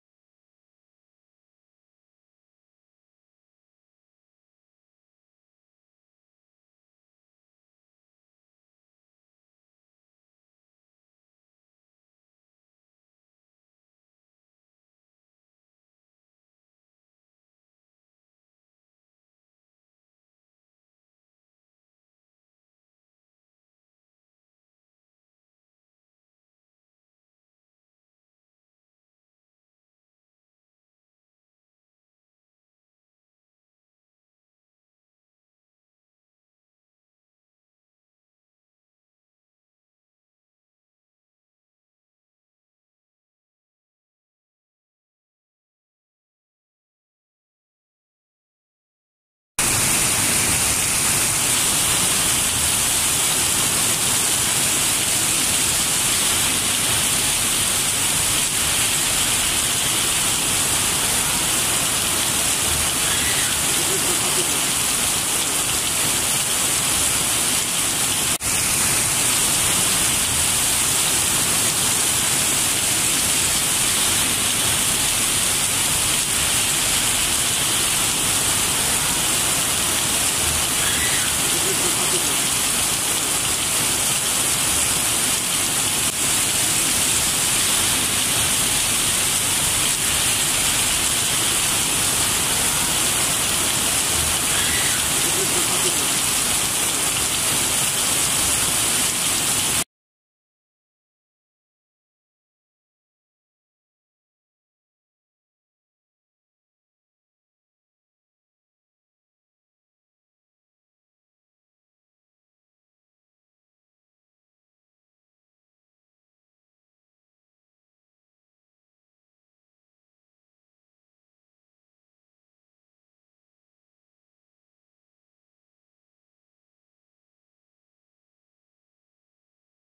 Praise be to God and thanks be to God Good rain today in Marrakech ☔☔☔
مريم أبوزاهر meriem abouzahar
أمطار الخير اليوم و لله الحمد بمراكش
مراكش

meriem abouzahar Good rain today, thank God, in Marrakech Marrakesh ☔☔☔☔☔☔☔☔☔ 😊😊😊